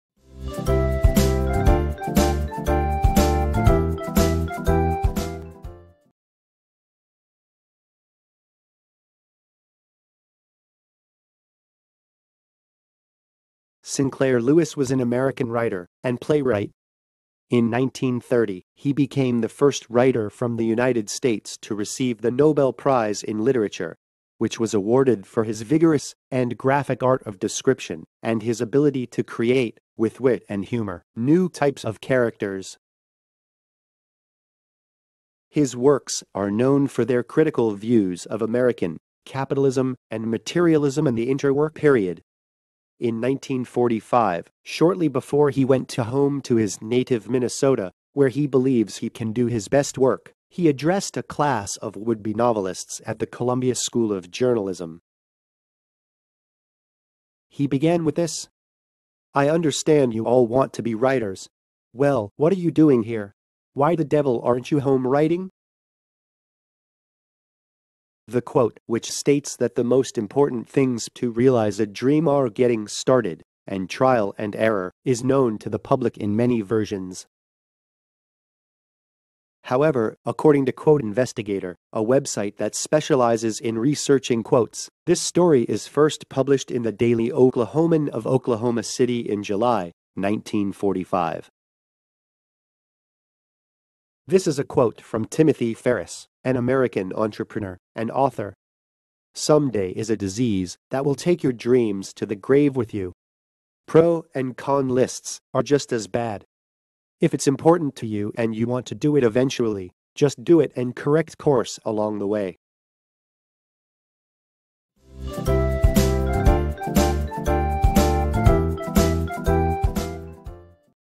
Short Story: Someday Disease - Why the devil aren't you home writing? (Sinclair Lewis)
The quote, which states that the most important things to realize a dream are getting started and trial and error, is known to the public in many versions. However, according to 'Quote Investigator', a website that specializes in researching quotes, this story is first published in 'The Daily Oklahoman' of Oklahoma City in July 1945.
This is a quote from Timothy Ferriss, an American entrepreneur and author.
"'Someday' is a disease that will take your dreams to the grave with you. Pro and con lists are just as bad. If it's important to you and you want to do it 'eventually', just do it and correct course along the way."